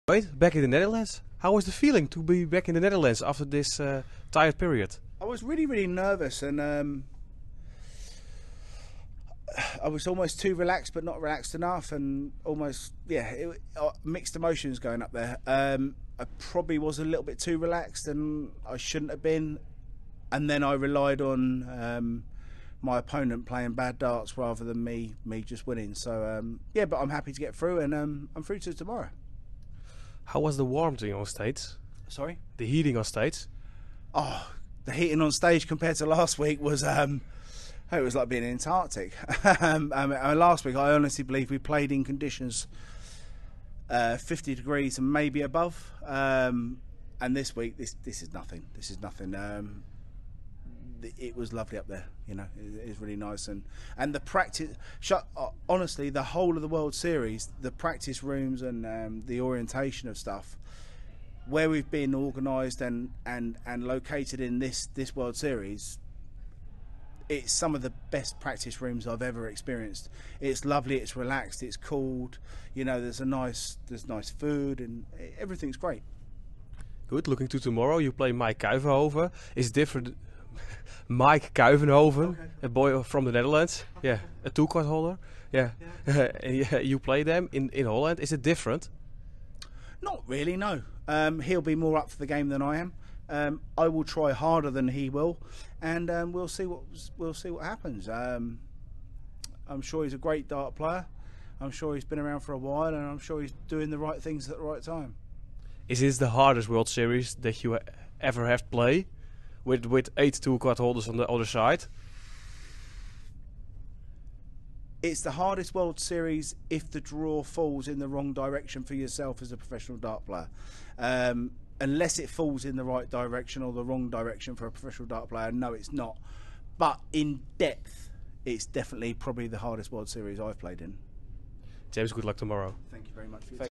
James Wade: 'In depth it is the hardest World Series I have played in' | Dutch Darts Masters 2022
Dit weekend (24-25 juni) wordt in de Ziggo Dome te Amsterdam het derde World Series-evenement van het jaar gespeeld, de Dutch Darts Masters. Vrijdagavond won James Wade in de eerste ronde met 6-2 van Jeffrey de Zwaan. Na afloop sprak Darts Actueel met 'The Machine'!

Al het dartnieuws waaronder de Dutch Darts Masters is terug te vinden op onze website!